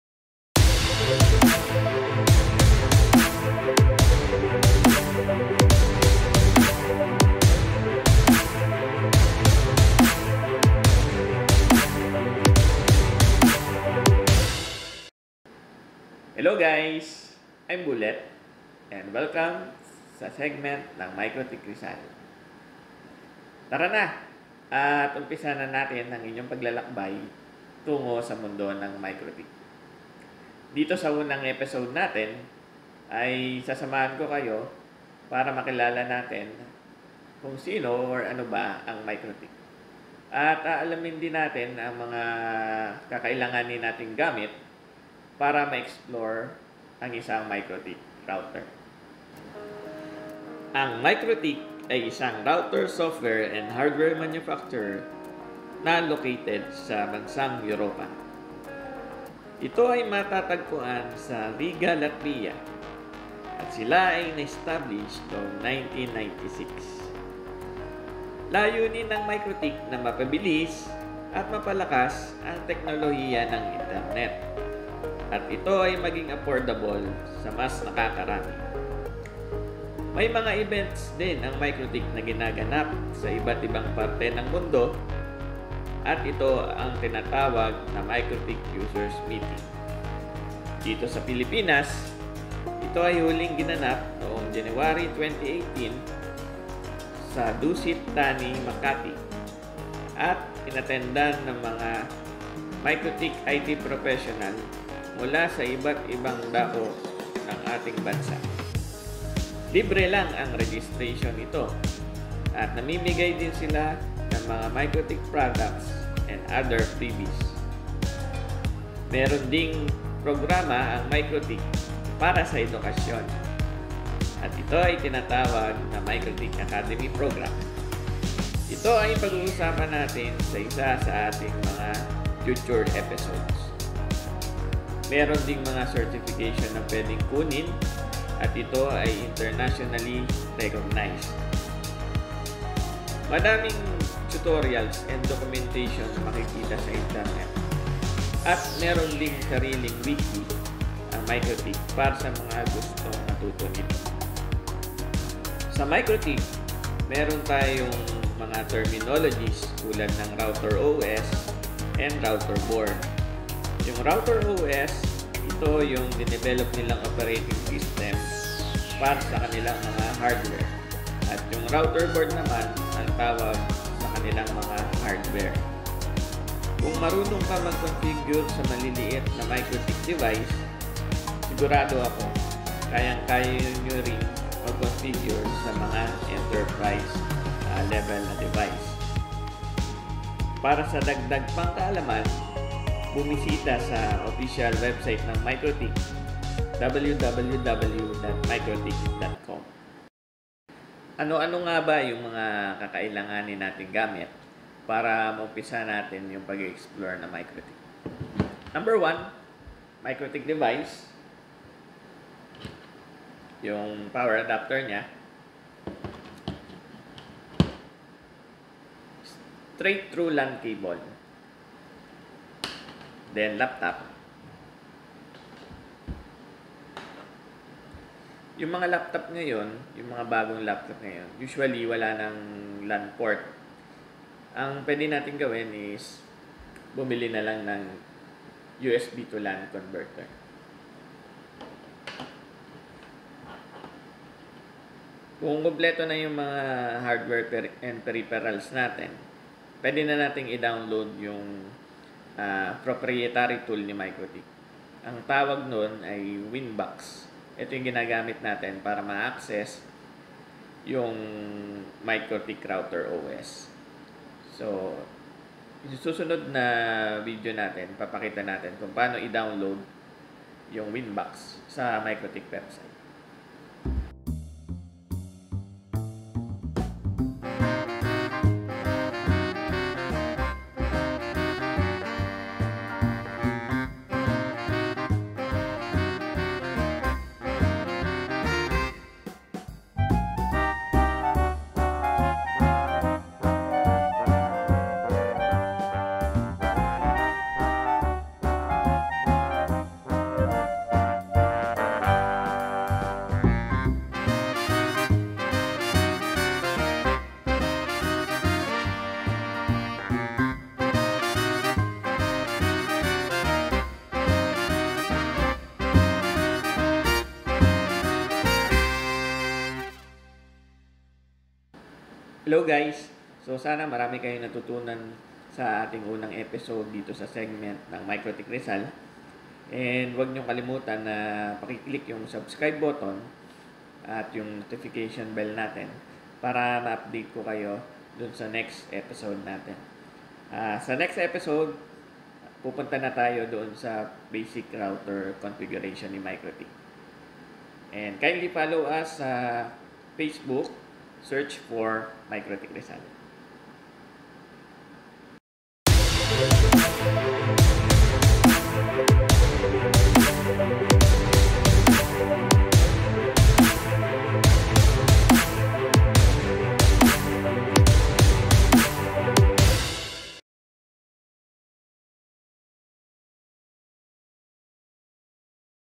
Introduction to MikroTik (Tagalog with English Subtitle)
Episode 1: Introduction to MikroTik (Tagalog with English Subtitle)

**In this Episode, we will introduce the MikroTik Brand and will learn the things that we will need in order to explore a MikroTik Device including how to download Winbox in the MikroTik Website.

Episode 1: Panimula Tungkol sa MikroTik (Tagalog with English Subtitle)
**Sa Unang Episode na ito, kikilalanin natin kung sino o ano ang MikroTik at ating aalamin ang mga gamit na kakailanganin para ating ma-explore ang isang MikroTik Device at kung papaano magdownload ng Winbox sa MikroTik Website.

MikroTik Rizal is Powered by Ingenu IT Solutions and Inquirinity Corporation.